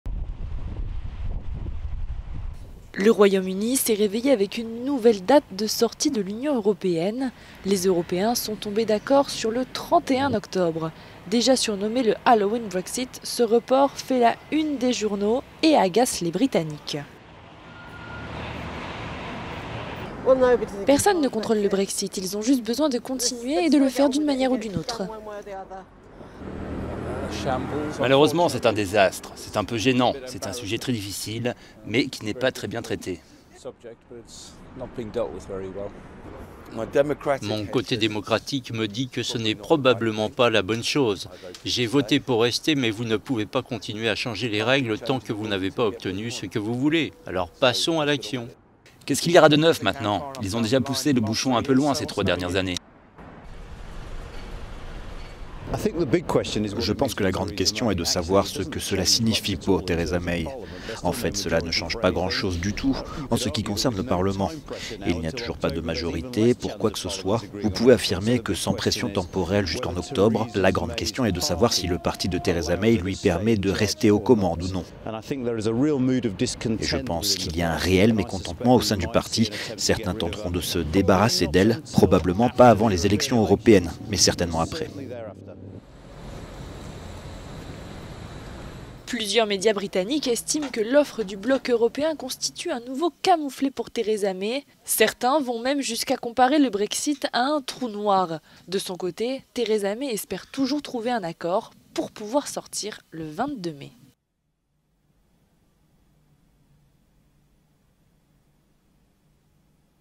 Les britanniques se réveillent avec un nouveau report du Brexit
Les Européens ont fini par se mettre d’accord sur une date de sortie de l’Union pour le Royaume-Uni, le 31 octobre. Un report qui fait la une des journaux et qui agace les britanniques.…